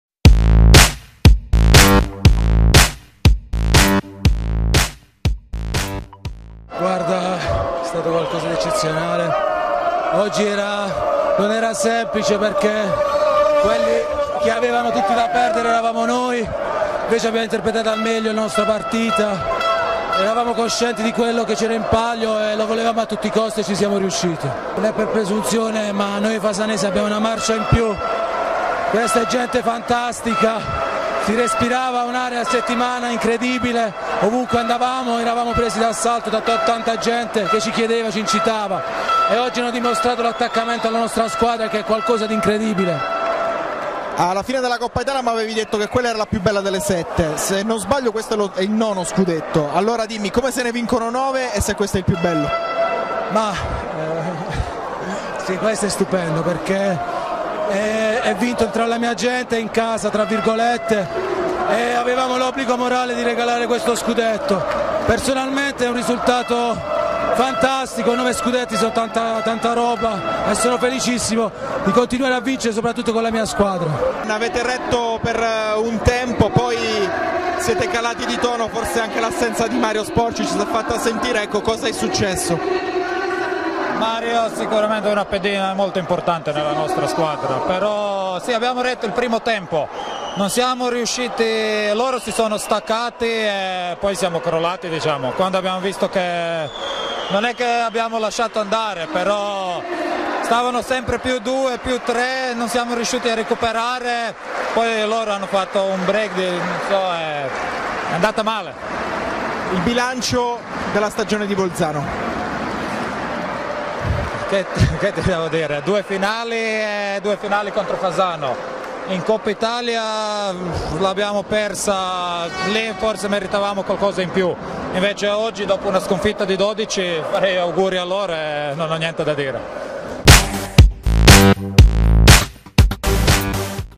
Serie A Maschile: Fasano campione d'Italia 2015/16
La Junior Fasano vince il secondo Scudetto della sua storia, battendo 31-19 in gara-2 di finale l'SSV Bozen. A fine gara le parole di Vito Fovio (Fasano) e Dean Turkovic (Bozen).